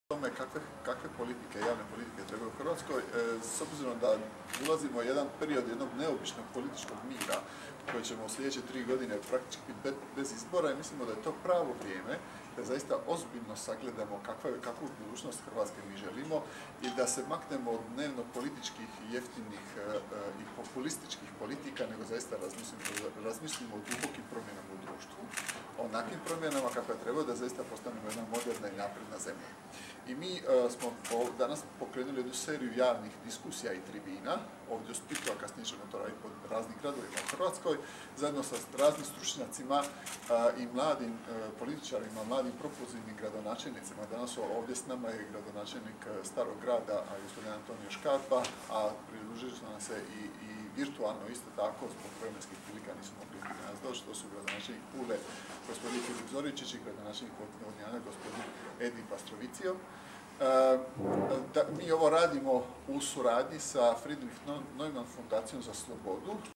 Izjava Puljka u okviru konferencije „Javno dobro i privatno poduzetništvo“
Split, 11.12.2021. - Konferencija za medije u okviru konferencije „Javno dobro i privatno poduzetništvo“. Na video snimci govori Ivica Puljak. video HINA/ Mario STRMOTIĆ/ ml